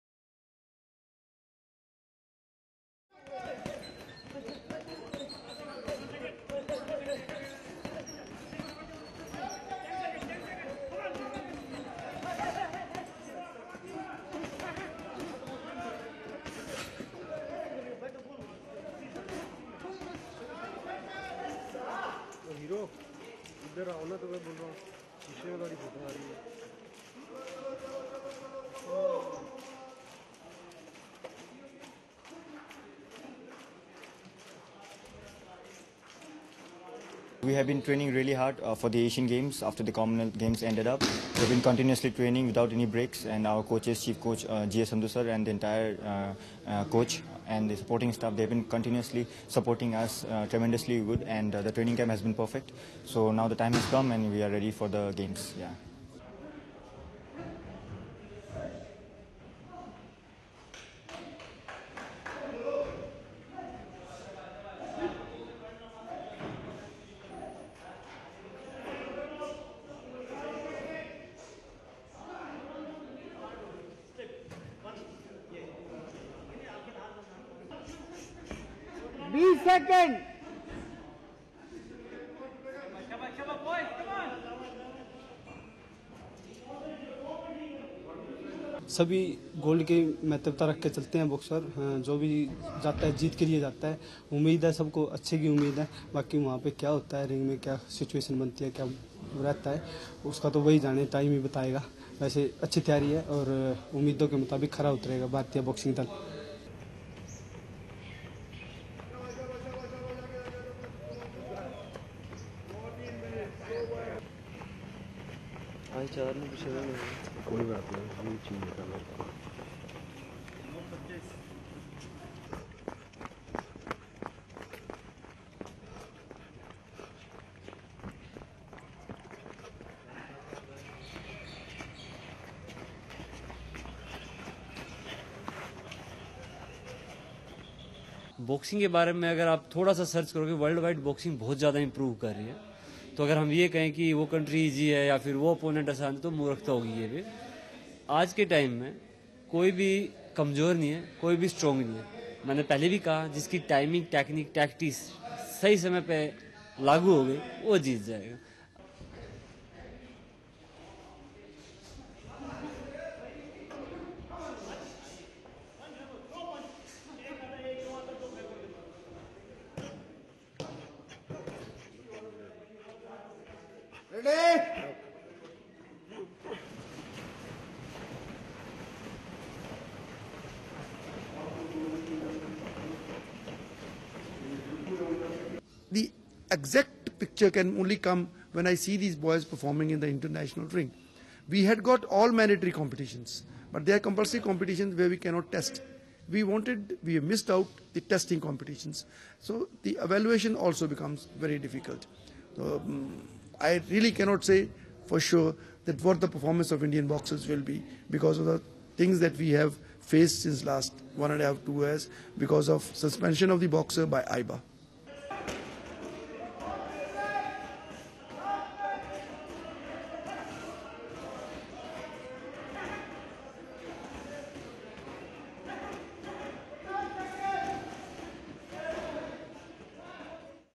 Indian boxers' sweat, eye gold at Incheon Asian Games
Indian boxers sweat it out in the final training camp in northern Patiala city ahead of their departure for the Asian Games in South Koera's Incheon hoping for rich haul of medals from the quadrennial event.